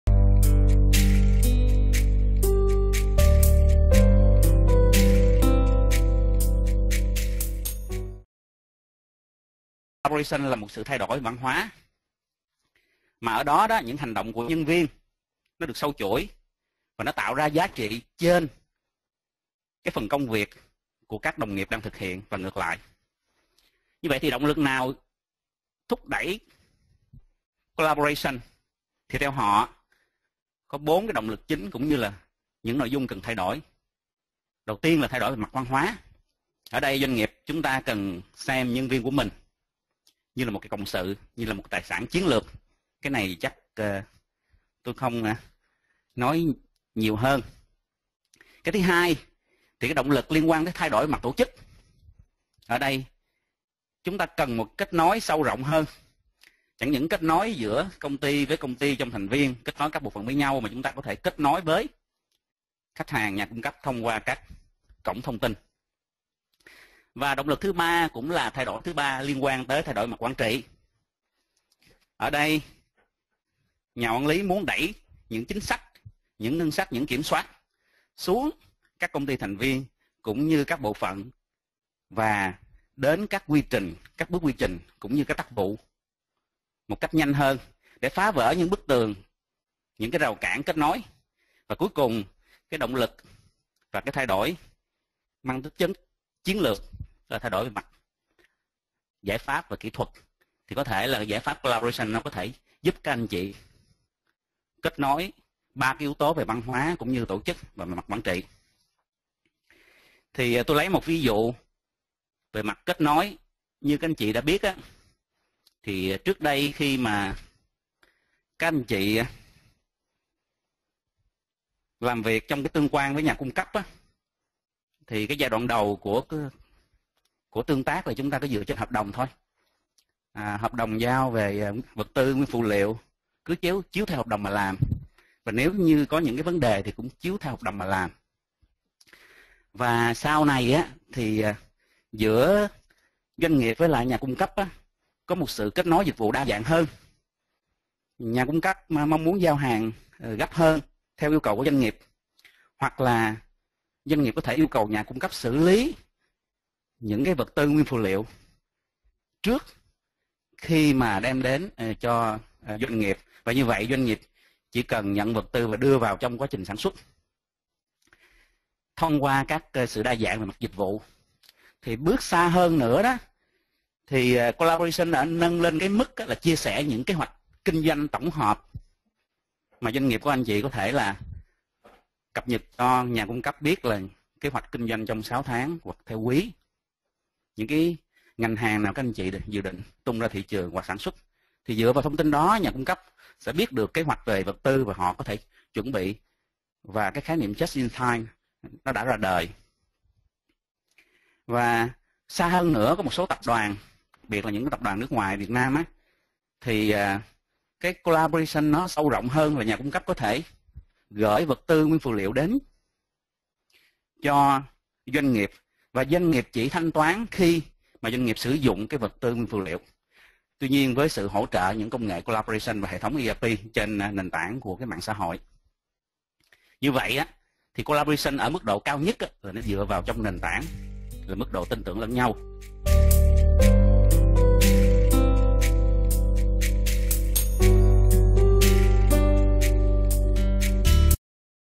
ERP Collaboration - part 1: How ERP System helps improve collaboration
There are 4 drivers of collaboration including Cultural change, Organisational change, Management change, Technological change. Of which technological change is the strategic driver help to connect the others. As an example of technological change, ERP system has helped improve collaboration in manufacturing companies.

For more information about Infor ERP system, please contact